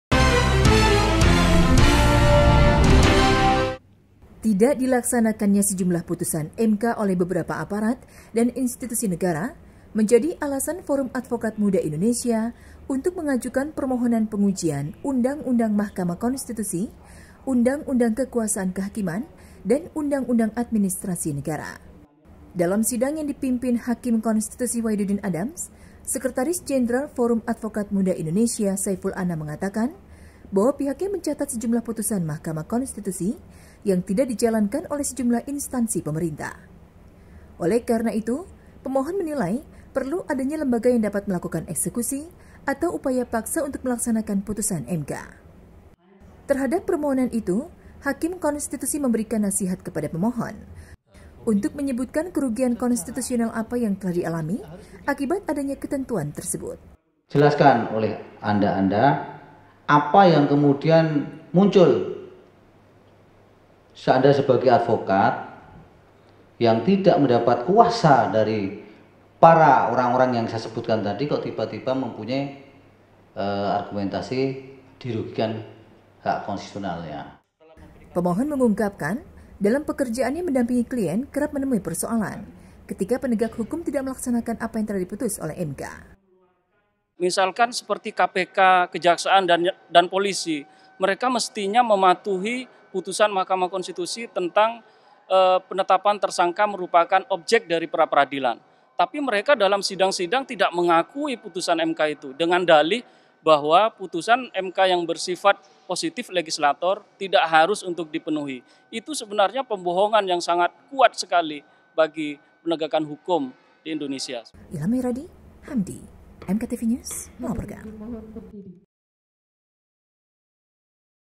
-Advokat Muda Minta MK Tegaskan Kewajiban Mematuhi Putusan MK
Mahkamah Konstitusi (MK) menggelar sidang perdana uji materiil UU No 8 Tahun 2011 tentang Mahkamah Konstitusi (UU MK), UU Nomor 48 Tahun 2009 tentang Kekuasaan Kehakiman (UU Kekuasaan Kehakiman), dan UU Nomor 30 Tahun 2014 tentang Administrasi Pemerintahan (UU Administrasi Pemerintahan), Selasa (22/11) di ruang sidang MK. Pemohon perkara teregistrasi Nomor 105/PUU-XIV/2016  tersebut adalah Forum Advokat Muda Indonesia (FAMI).

Para pemohon mengajukan uji materiil Pasal 10 ayat (1) dan Pasal 47 UU MK, Pasal 29 ayat (1) UU Kekuasaan Kehakiman, dan Pasal 7 ayat (2) huruf l UU Administrasi Pemerintahan. Menurut pemohon, pasal-pasal tersebut terkait dengan kewajiban mematuhi putusan MK.

Pasal 10 ayat (1) UU MK menyatakan,

“Mahkamah Konstitusi berwenang mengadili pada tingkat pertama dan terakhir yang putusannya bersifat final untuk : a. Menguji Undang-Undang terhadap Undang-Undang Dasar Negara Republik Indonesia Tahun 1945; b. Memutus sengketa kewenangan lembaga Negara yang kewenangannya diberikan oleh Undang-Undang Dasar Negara Republik Indonesia Tahun 1945; c. Memutus pembubaran partai politik; dan d. Memutus perselisihan tentang hasil pemilihan umum”.

Pasal 47 UU MK menyatakan,

“Putusan Mahkamah Konstitusi memperoleh kekuatan hukum tetap sejak selesai diucapkan dalam sidang pleno terbuka untuk umum”;

Pasal 29 ayat (1) UU Kekuasaan Kehakiman menyatakan,

Pasal 7 ayat (2) huruf l UU Administrasi Pemerintahan menyatakan,

Pejabat Pemerintahan memiliki kewajiban: l. Mematuhi Putusan Pengadilan yang telah berkekuatan hukum tetap.

Salah satu Pemohon, Saeful Anam, menyatakan asas putusan MK adalah res judicata (putusan hakim harus dianggap benar). Selain itu, putusan MK juga bersifat res judicata pro veritate habetur (apa yang diputus hakim harus dianggap benar dan harus dilaksanakan).

Namun, menurutnya,  fakta di lapangan Putusan MK banyak yang bersifat non-excutiable (tidak dapat dijalankan). Pemohon menilai tidak cukup apabila hanya menekankan pada asas self respect dan kesadaran hukum kepada pihak manapun, baik pemerintah, pejabat publik, perseorangan, badan hukum, dan pihak lain yang terkait untuk melaksanakan putusan MK.“Untuk itu, perlu upaya paksa dalam usaha melaksanakan Putusan MK. Mesti tercantum secara langsung melalui pasal-pasal yang berkaitan kekuatan mengikat Putusan MK,” jelasnya dalam sidang yang dipimpin Hakim Konstitusi Wahiduddin Adams.Adapun Wahiduddin meminta Pemohon mempertajam legal standing-nya dengan memperjelas kerugian konstitusional yang dialami. “Misal FAMI ini apa, tujuannya apa. Kenapa bisa concern dalam hal seperti ini?” jelasnya.

(ars/lul)